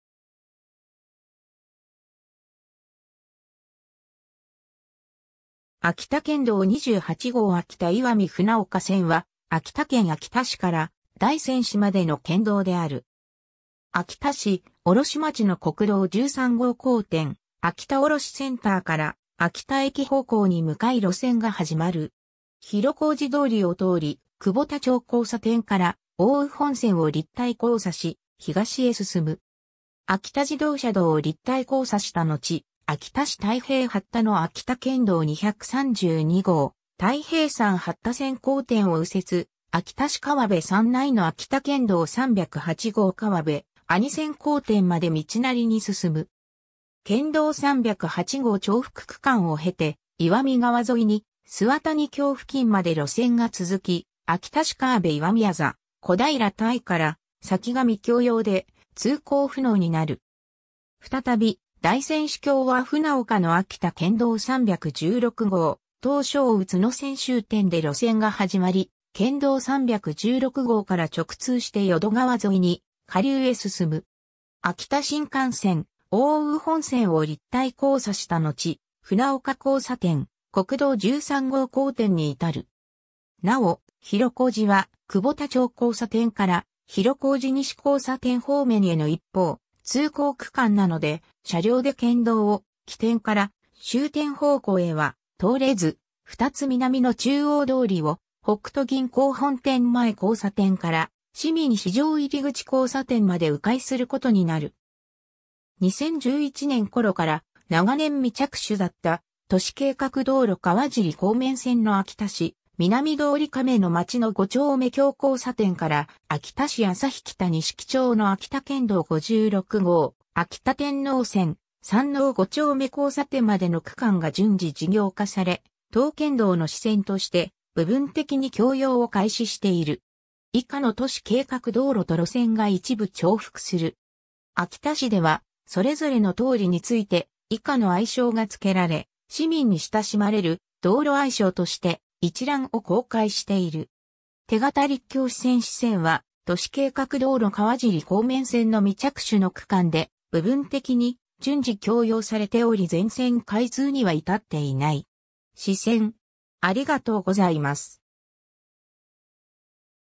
秋田県道28号秋田岩見船岡線
秋田県道28号秋田岩見船岡線（あきたけんどう28ごう あきたいわみふなおかせん）は、秋田県秋田市から大仙市までの県道（主要地方道）である。
 秋田市卸町の国道13号交点、秋田卸センターから秋田駅方向に向かい路線が始まる。
広小路通りを通り、久保田町交差点から奥羽本線を立体交差し、東へ進む。
秋田自動車道を立体交差したのち、秋田市太平八田の秋田県道232号太平山八田線交点を右折、秋田市河辺三内の秋田県道308号河辺阿仁線交点まで道なりに進む。
県道308号重複区間を経て、岩見川沿いに岨谷峡付近まで路線が続き、秋田市河辺岩見字小平岱から先が未供用で通行不能になる。
 再び大仙市協和船岡の秋田県道316号唐松宇津野線終点で路線が始まり、県道316号から直通して淀川沿いに下流へ進む。
秋田新幹線・奥羽本線を立体交差したのち、船岡交差点・国道13号交点に至る。
 なお、広小路は久保田町交差点から広小路西交差点方面への一方通行区間なので、車両で県道を起点から終点方向へは通れず、2つ南の中央通りを北都銀行本店前交差点から市民市場入口交差点まで迂回することになる。
 2011年ころから、長年未着手だった都市計画道路川尻広面線の秋田市南通亀の町の五丁目橋交差点から秋田市旭北錦町の秋田県道56号秋田天王線・山王五丁目交差点までの区間が順次事業化され、当県道の支線として部分的に供用を開始している。
 以下の都市計画道路と路線が一部重複する。
 秋田市ではそれぞれの通りについて以下の愛称がつけられ、『市民に親しまれる道路愛称』として一覧を公開している。
 手形陸橋 支線 支線は、都市計画道路川尻広面線の未着手の区間で、部分的に順次供用されており全線開通には至っていない。
 支線